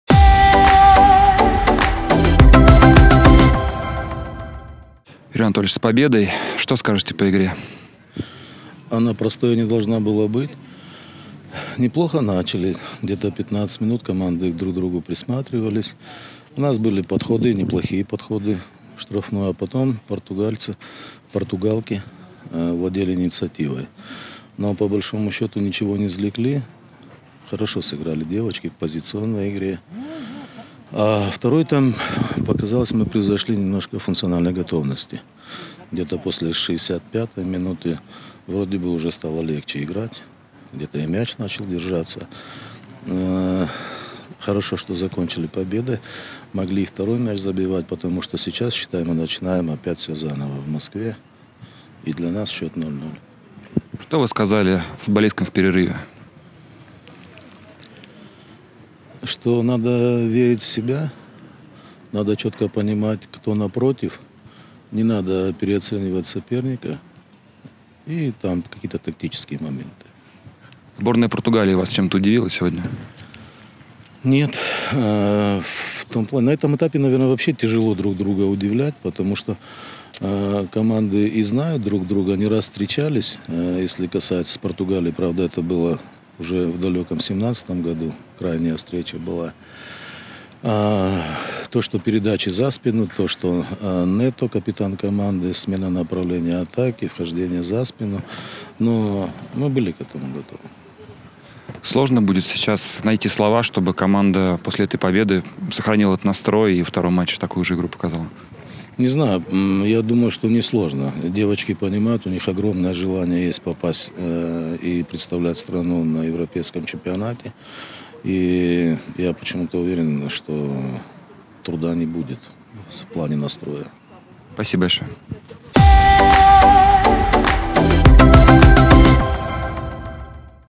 Юрий Красножан: "В Москве надо начинать всё заново"
Главный тренер сборной России подвел итог первого стыкового матча против Португалии.